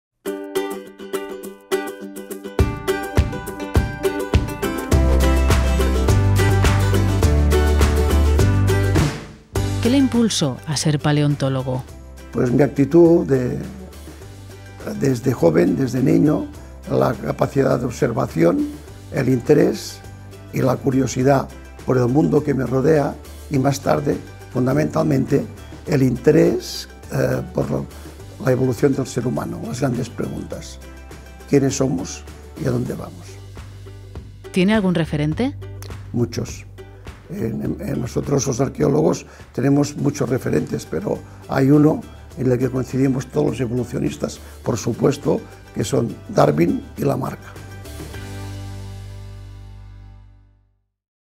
20. ¿Qué le impulsó a ser paleontólogo?
Fundación Palarq y Eudald Carbonell abren una ventana a la ciencia para acercar la evolución humana a todos los públicos.

El codirector de Atapuerca responde a preguntas relacionadas con la paleontología humana y resolverá las dudas sobre nuestra evolución en la web de la Fundación.